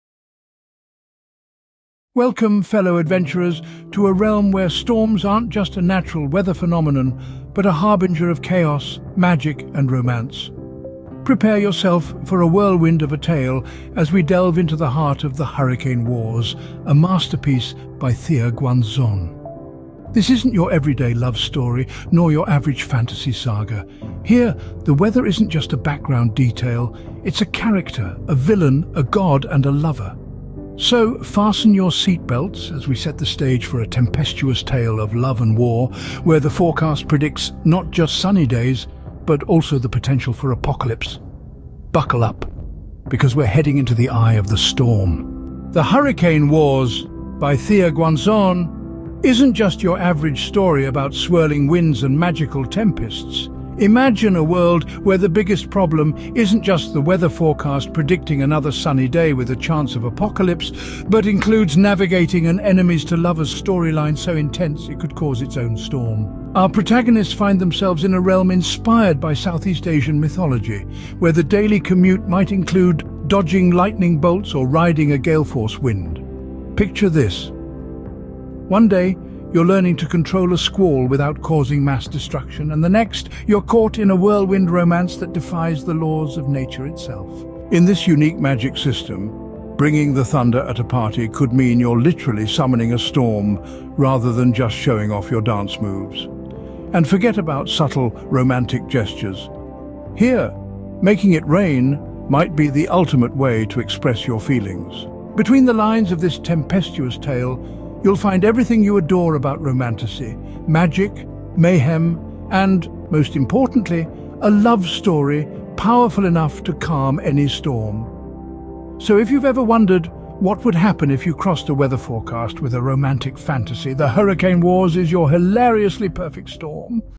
Hurricane Wars - Book Summary
Dive into the eye of the storm with our gripping summary of 'Hurricane Wars'! 🌪️ This book takes readers on a thrilling journey through the most powerful storms on Earth, exploring the science behind hurricanes, the devastating impacts they can have on communities, and the heroic efforts of those who stand in their path. From historical storms that have shaped our understanding of weather to the latest in storm prediction and management, 'Hurricane Wars' is a testament to human resilience and ingenuity in the face of nature's fury. Whether you're a weather enthusiast, a student of environmental science, or simply looking for an exhilarating read, our summary of 'Hurricane Wars' offers key insights and takeaways that will leave you both informed and inspired. Watch now to unravel the tales of courage, tragedy, and triumph in the battle against hurricanes."